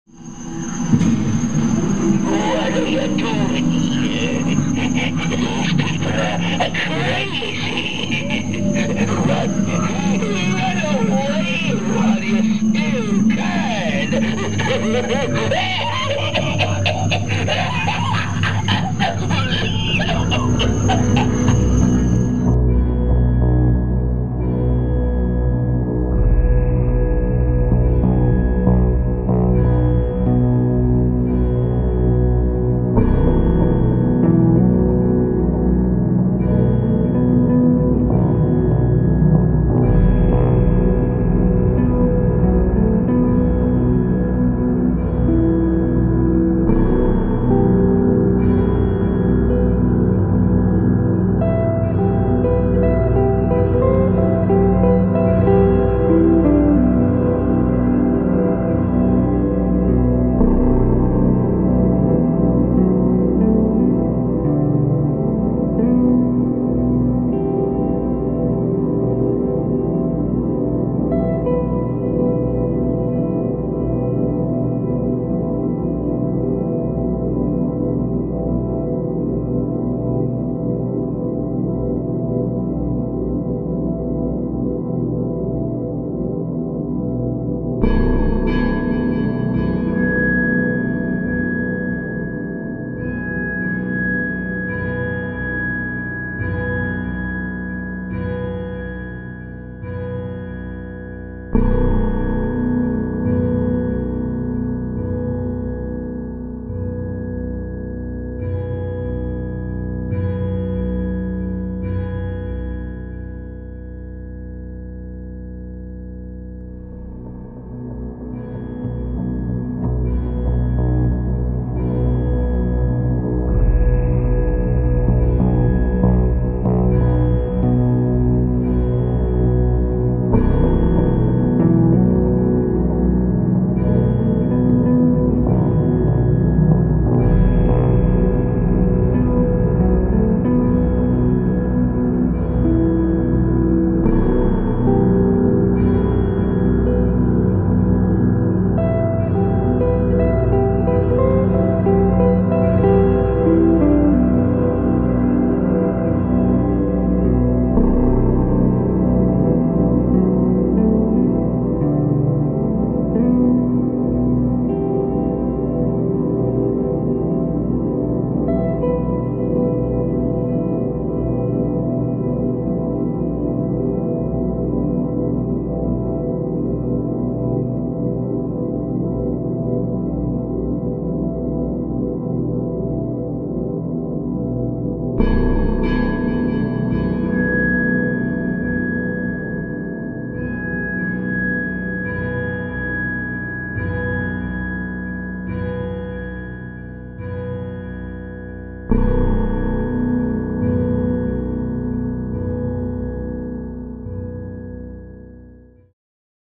Halloween 2014 Haunt
The weather held me back from a full display, but the trick or treaters had fun none the less!